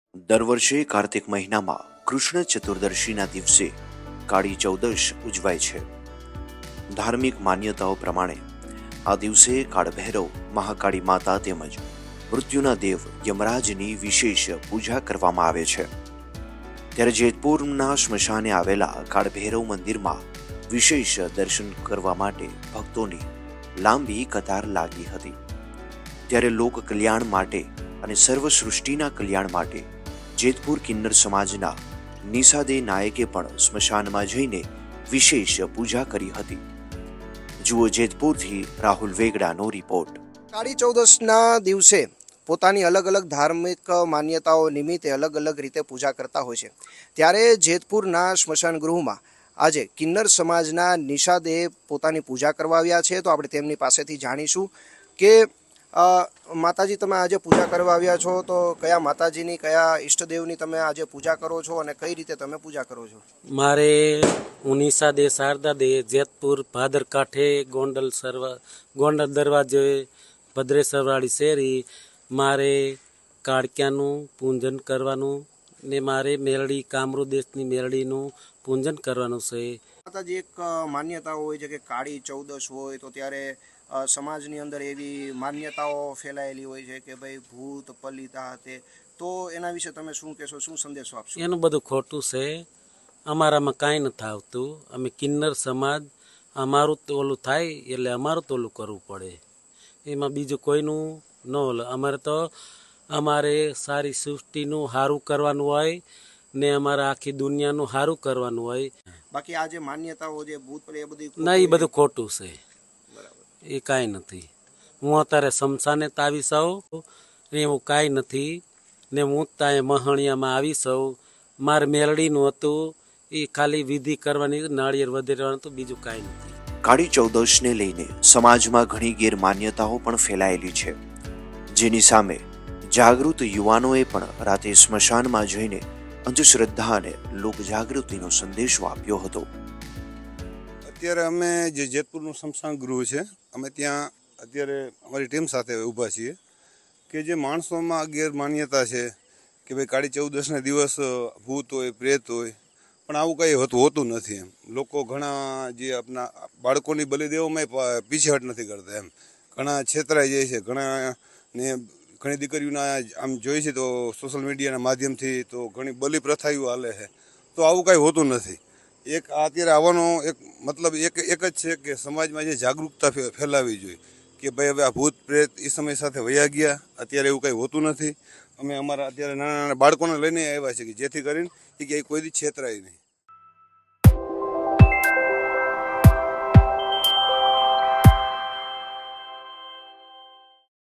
Jetpur | કાળી ચૌદશે સ્મશાનમાં કઈ રીતે પૂજા થાય । kali chaudas puja | Mumbai Samachar
Unveiling the Mystical Kali Chaudas Puja in Smashan | Hinduism Facts

Join us as we explore the significance of Kali Chaudas Puja, performed on the eve of Diwali, in the sacred Smashan (cremation ground).

In this video, we delve into:

Hindu mythology surrounding Goddess Kali
Significance of Smashan in Hinduism
Rituals and practices of Kali Chaudas Puja
Importance of worshiping the Divine Feminine

Kali Chaudas marks the victory of Goddess Kali over evil forces.
Smashan is considered a sacred space for spiritual growth.
Kali Puja is performed to ward off negative energies.

Watch now and discover the mystical world of Hinduism!